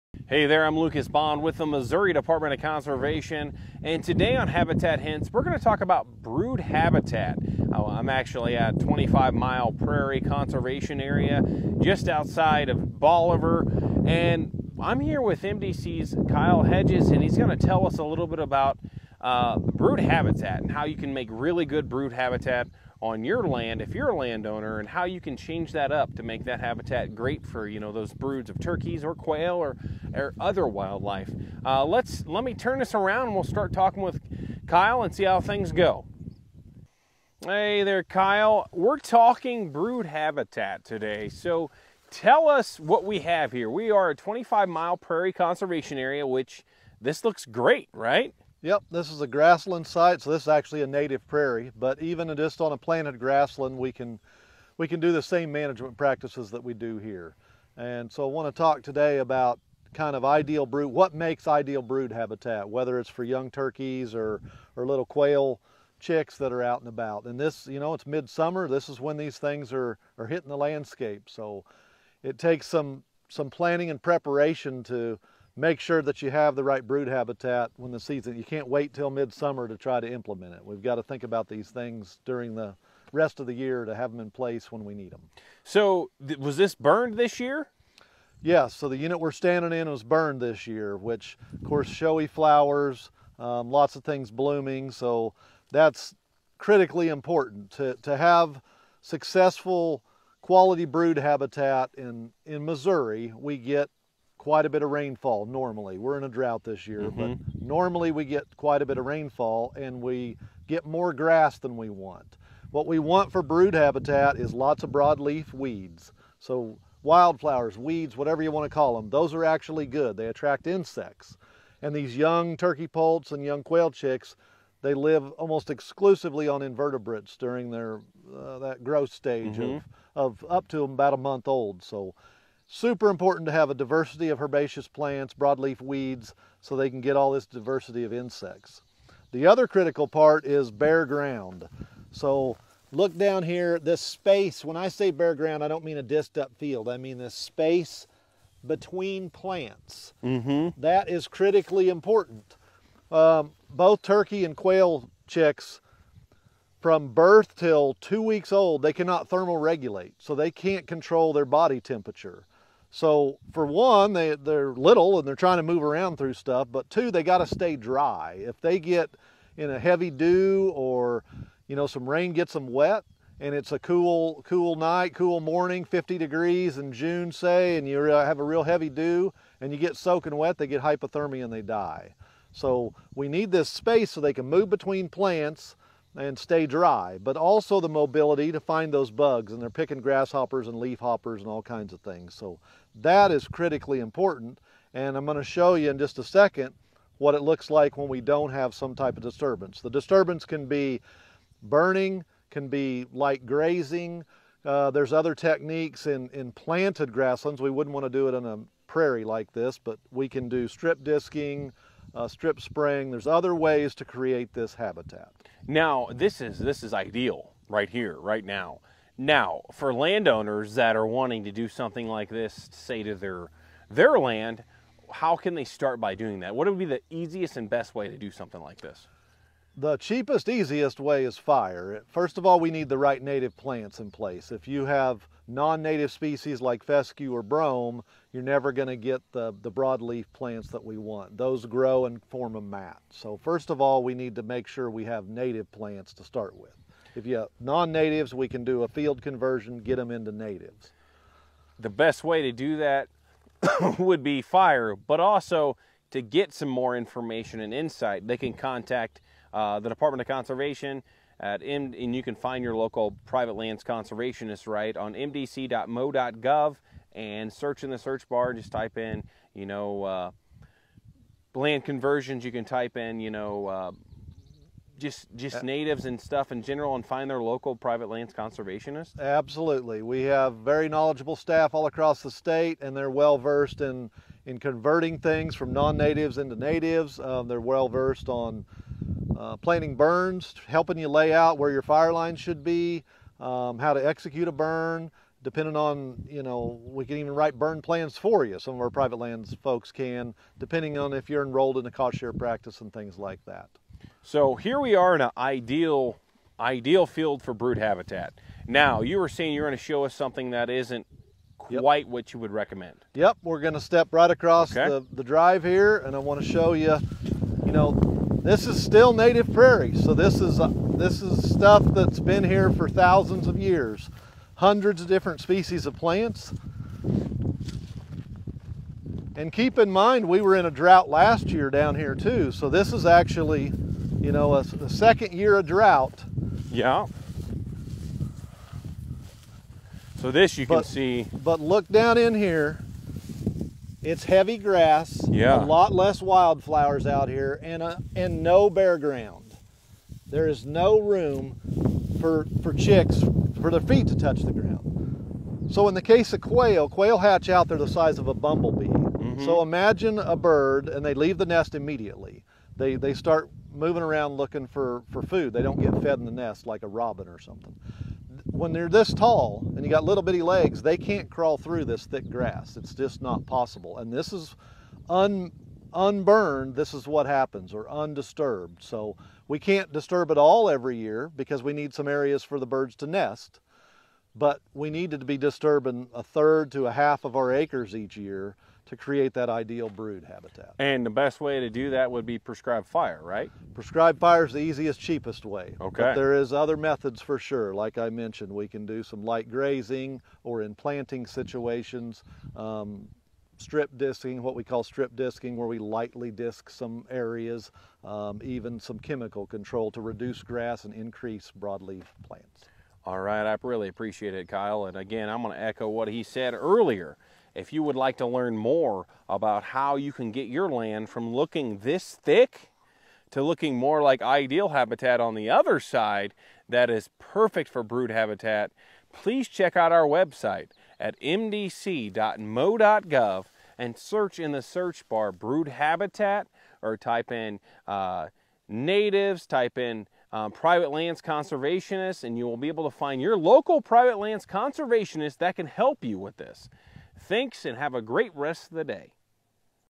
Habitat Hints - Brood Habitat
07-25-23 -- MDC's Kyle Hedges talks about what makes good brood habitat. He also shows us what good turkey and quail brood habitat looks like in this week's Habitat Hints. Learn more in the video.